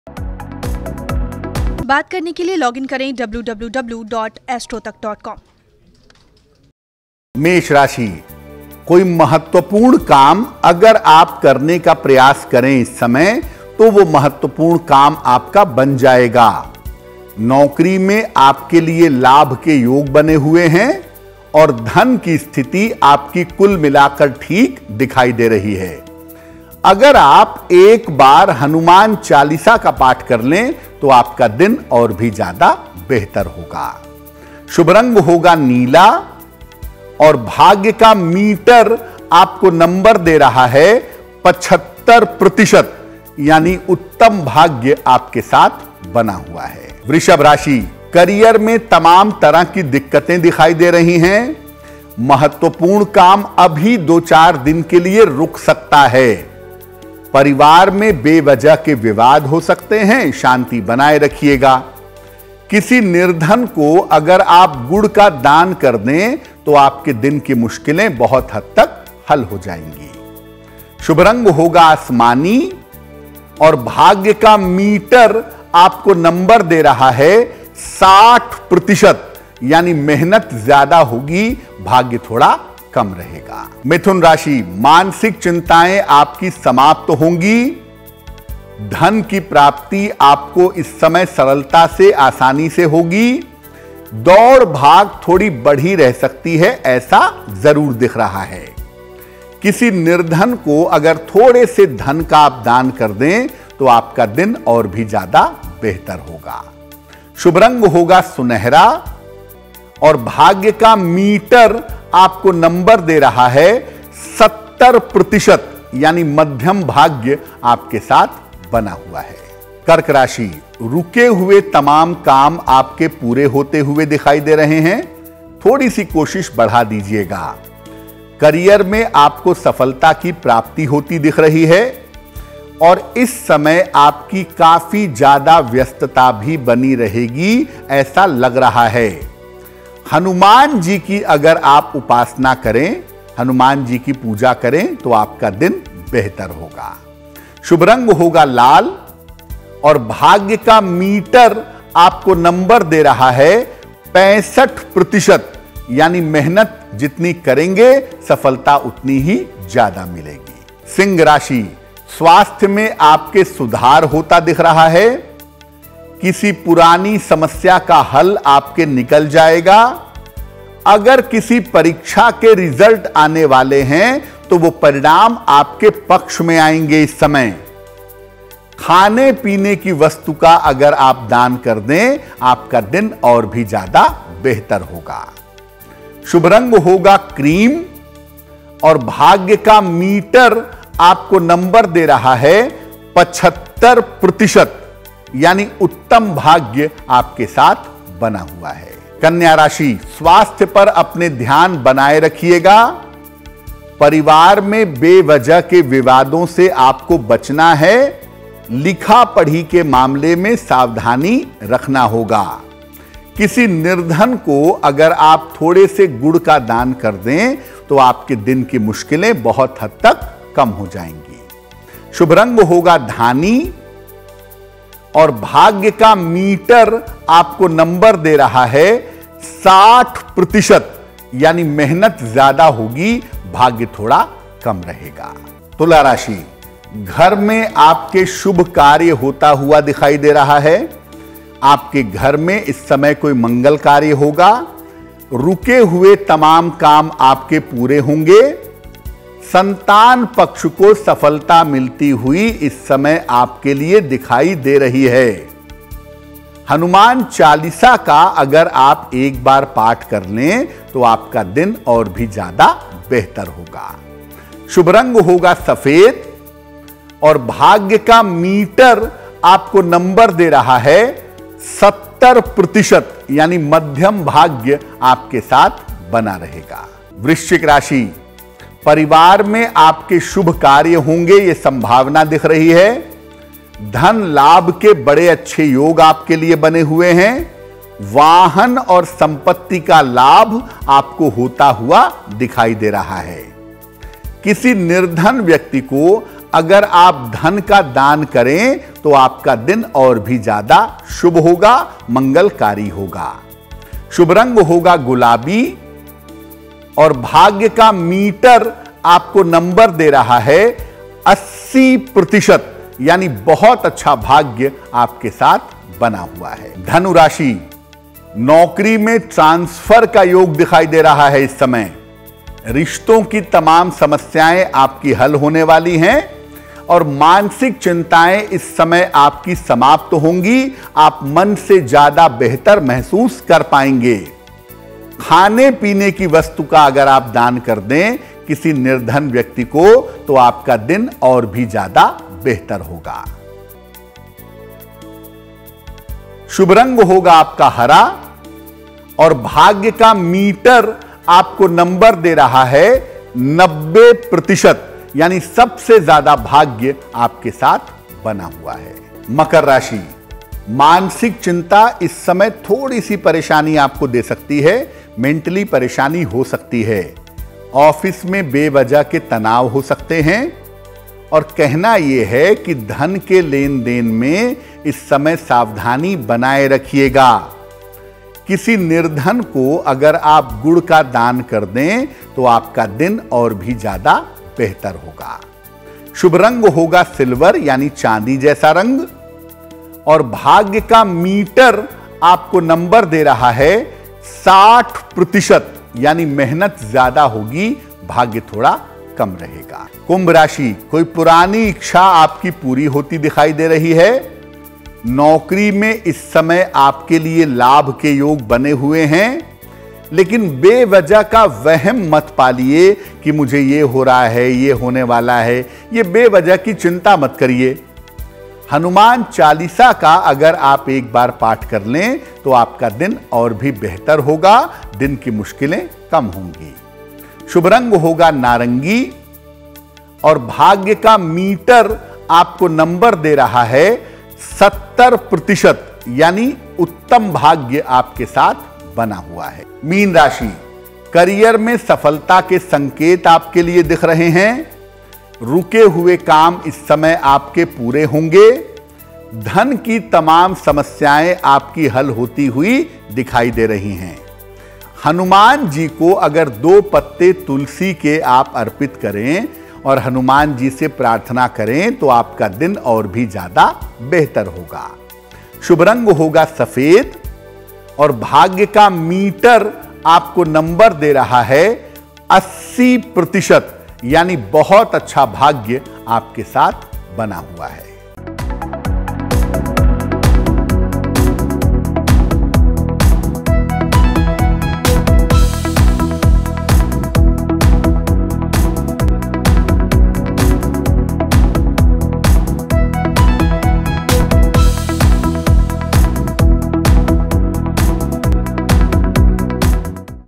06 September | शैलेंद्र पांडेय की भविष्यवाणी | Astro Tak
शैलेंद्र पांडेय की भविष्यवाणी- 06 September : जानिए अपने दिन का राशिफल साथ ही कौन सा रंग है आपके लिए शुभ और कितने प्रतिशत भाग्य देगा आपका साथ...सिर्फ शैलेंद्र पांडेय की भविष्यवाणी में...